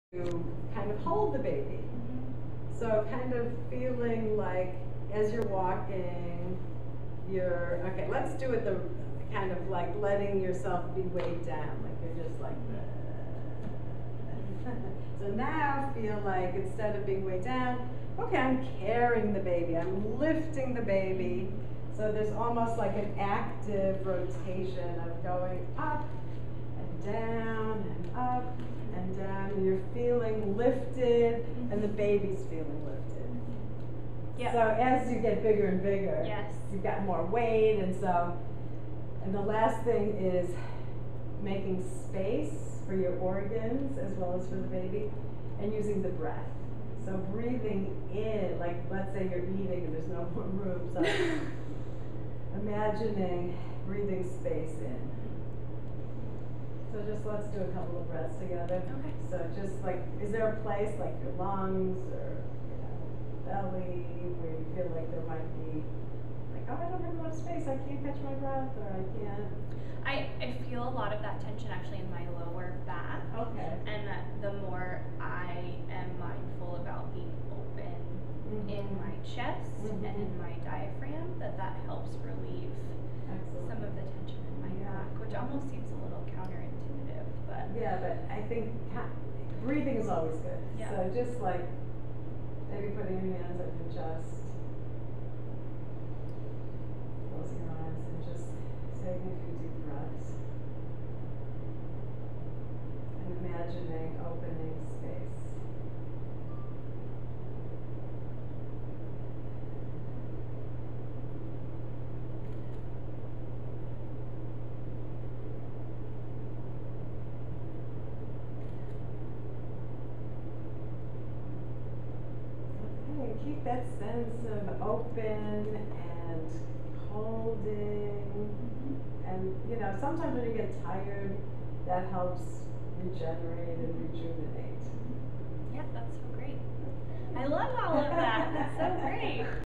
Supporting and Making Space for the Baby
"Throughout the pregnancy, the baby needs space in which to grow." Attention to certain movement patterns support the expectant mother in providing this space and support.

Loman, S. (2016) Judith S. Kestenberg's dance/movement therapy legacy: Approaches with pregnancy, young children, and caregivers. American Journal of Dance Therapy, DOI 10.100/s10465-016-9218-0